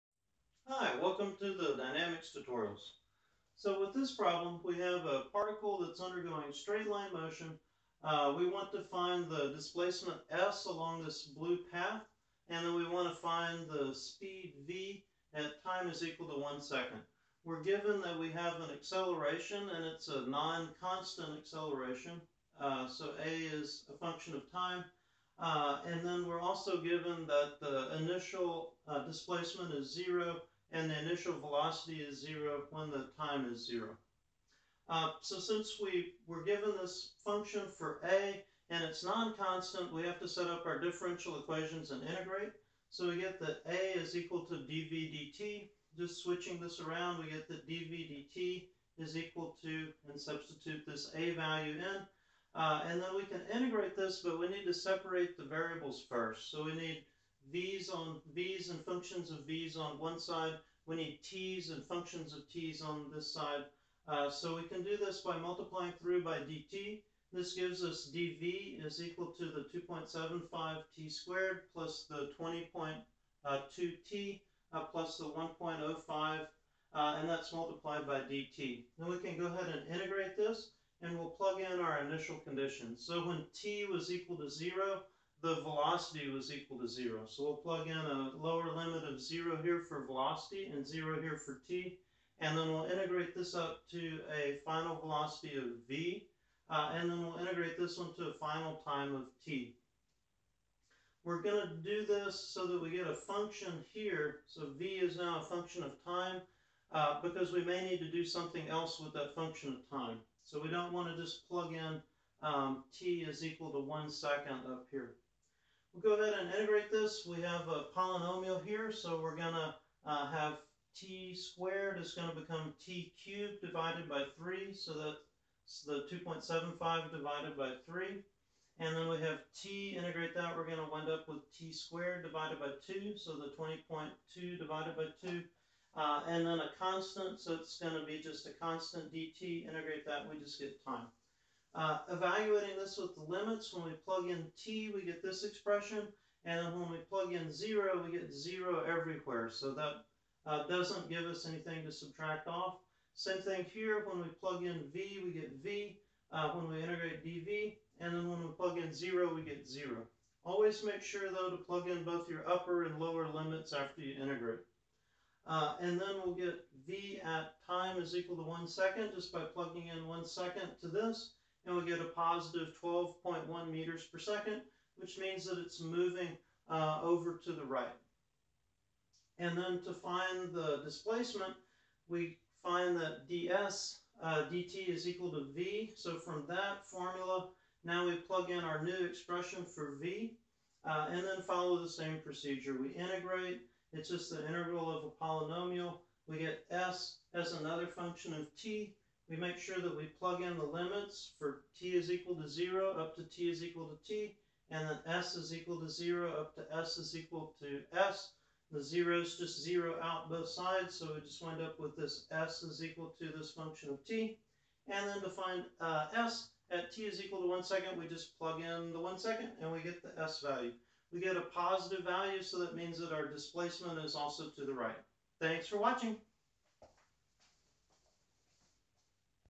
Dynamics: Rectilinear Motion
An example problem with rectilinear motion (kinematics) is solved in which the acceleration is a function of time.